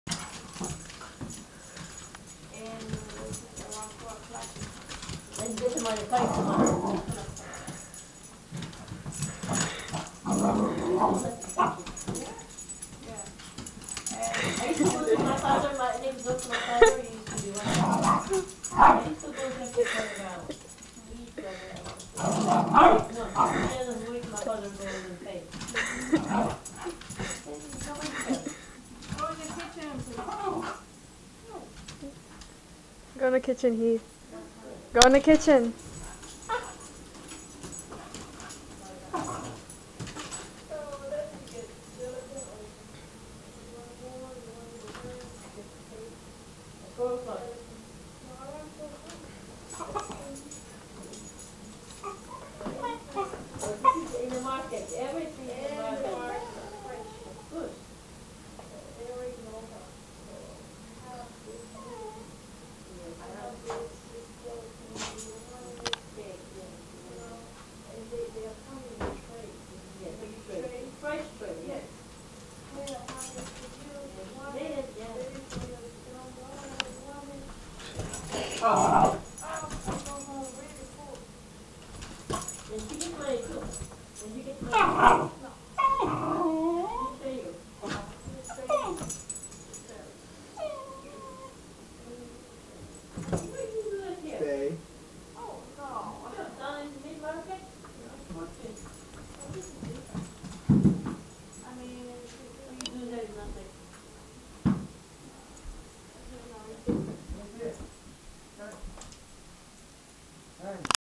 Pepper BARKING & CRYING.........
This is the frist time she bark at my brother..
She never barks.. she always quiet.